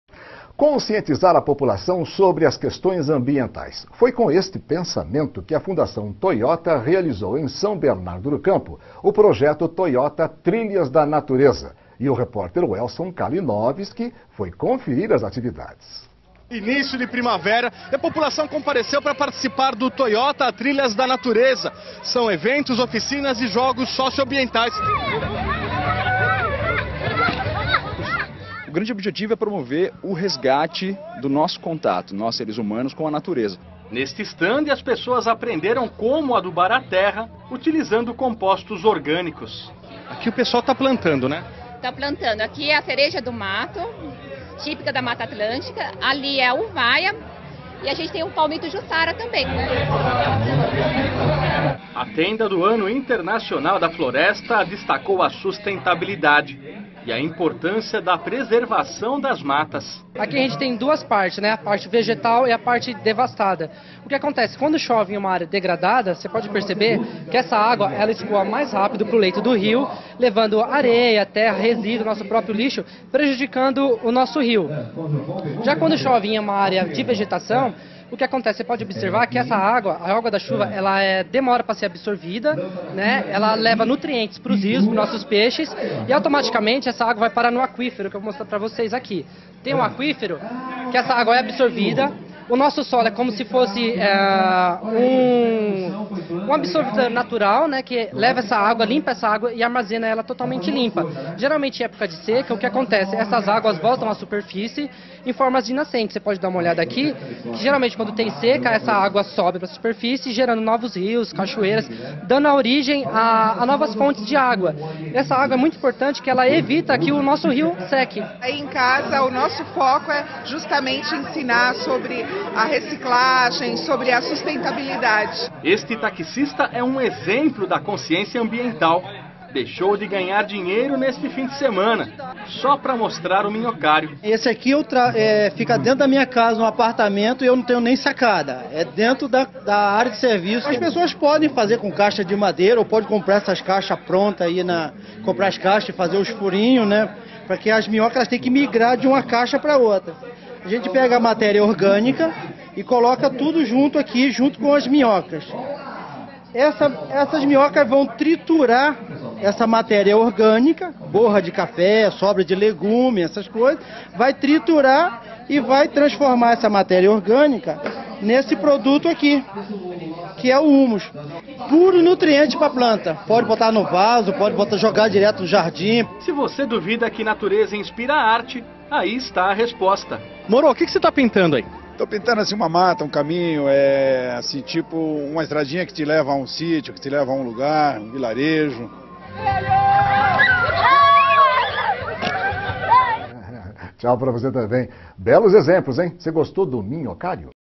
Trilhas da natureza leva cultura a São Bernardo do Campo
Um dia inteiro de oficinas, jogos socioambientais, peças teatrais, atividades circenses e shows. Tudo para conscientizar a população sobre as questões ambientais.